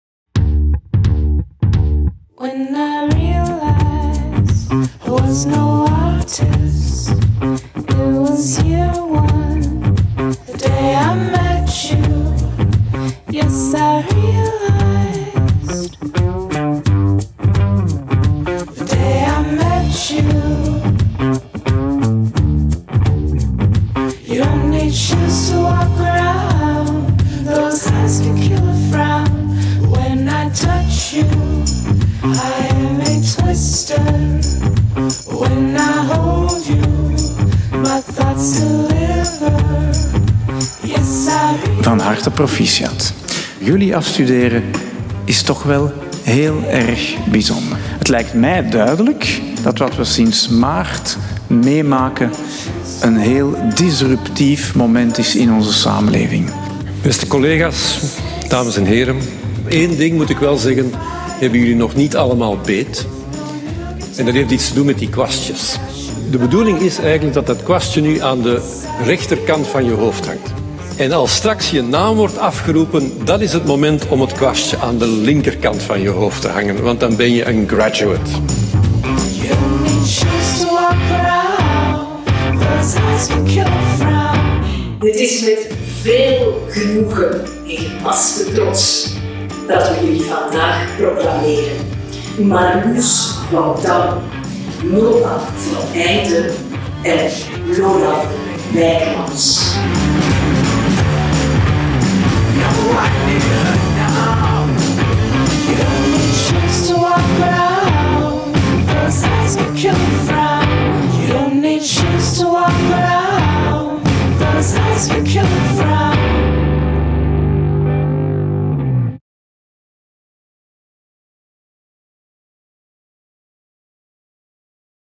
Aftermovie proclamaties geschiedenis, mpc en wijsbegeerte 2020
Kijk terug op de proclamaties van academiejaar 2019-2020 van de masters in de geschiedenis, meertalige professionele communicatie en wijsbegeerte.